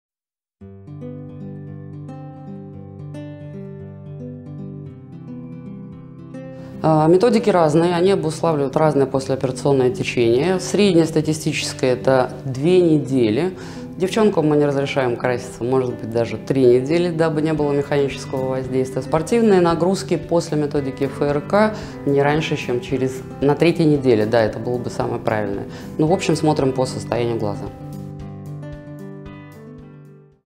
Спорт после лазерной коррекции зрения.
Через какое время после лазерной коррекции зрения можно заниматься спортом?

На вопрос отвечает Ниличева Екатерина Сергеевна, врач-офтальмохирург клиники «Эксимер» в Санкт-Петербурге.